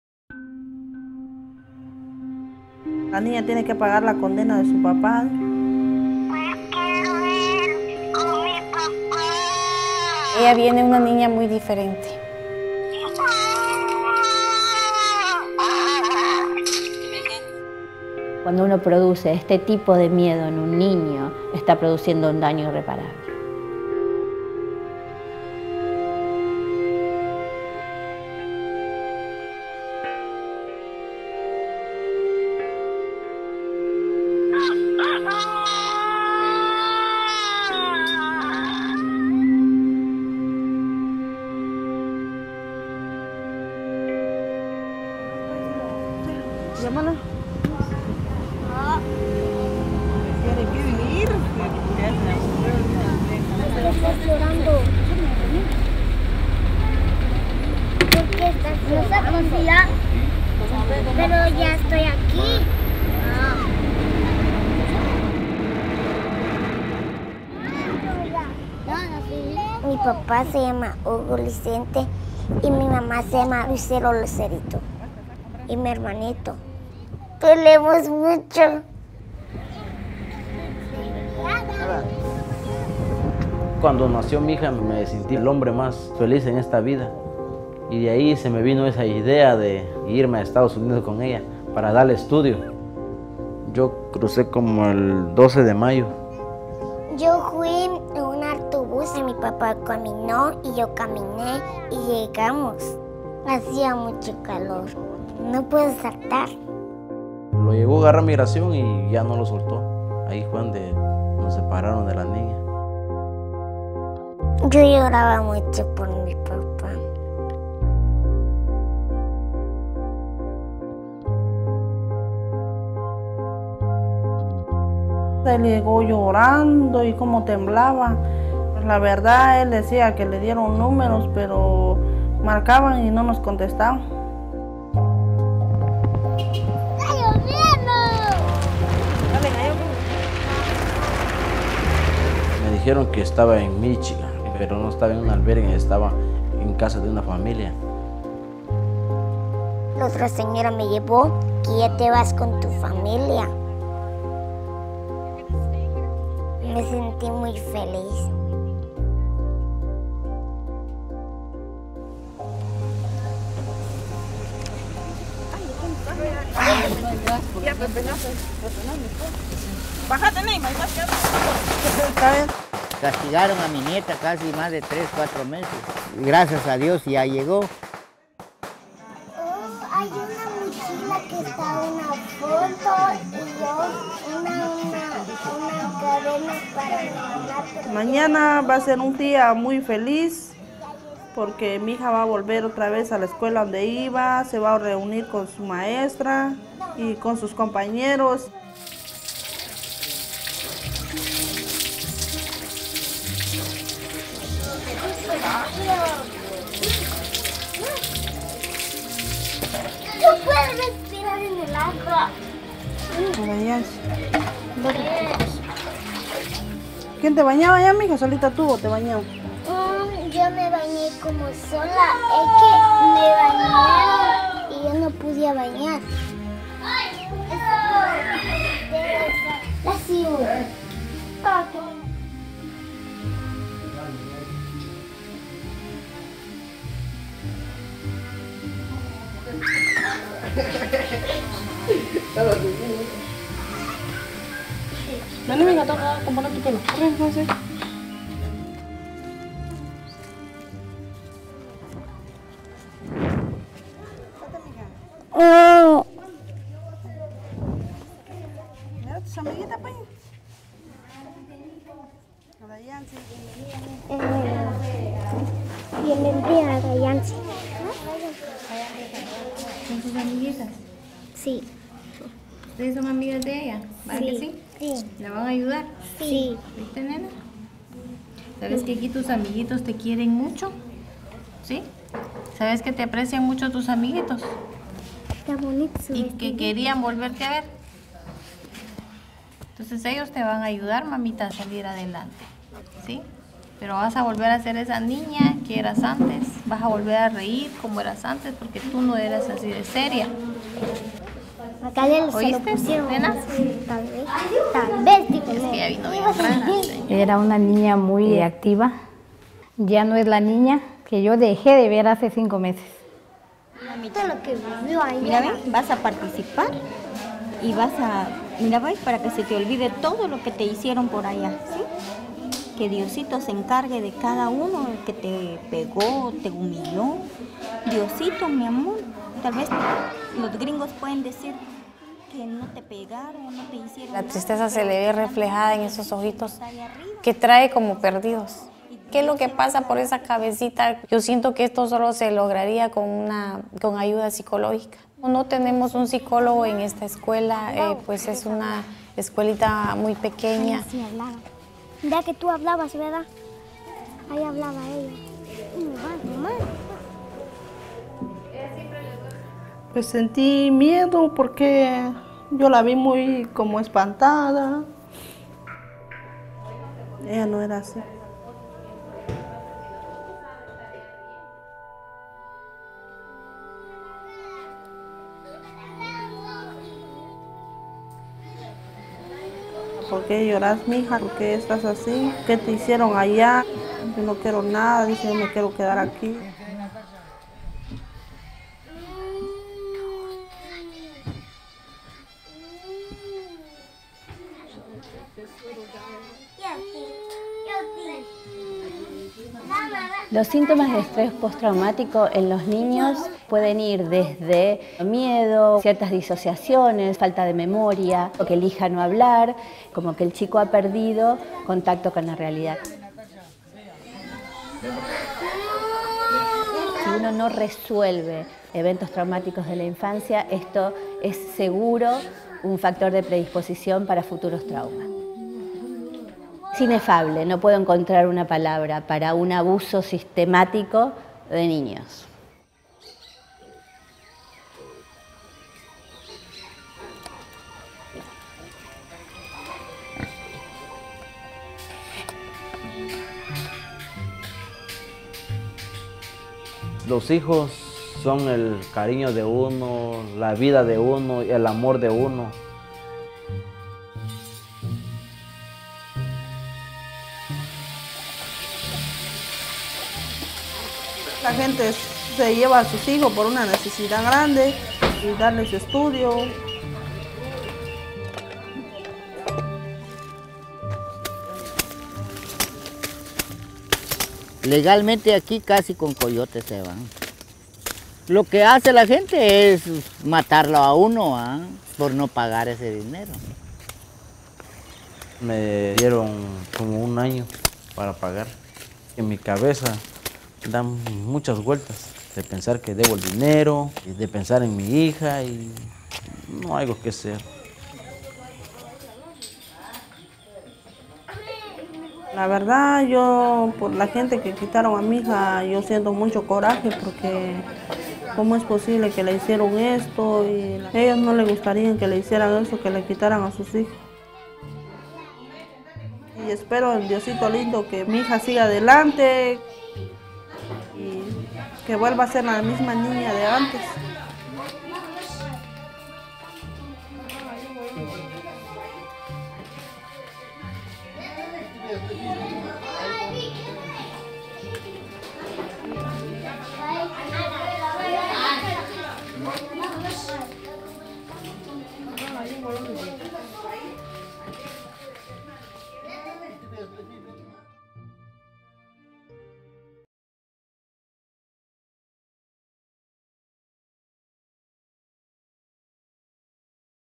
The legacy of the 'zero tolerance' policy: traumatized children with no access to treatment
Adayanci Pérez is one of more than 2,500 children that were separated from their families at the border by the U.S. government as part of the Zero Tolerance policy. This 6-year-old Guatemalan girl was away from her family for three and a half months. Her parents say that she has not been the same since her return. Before going back, she was diagnosed with PTSD. There are still 400 children that haven’t been reunited with their families.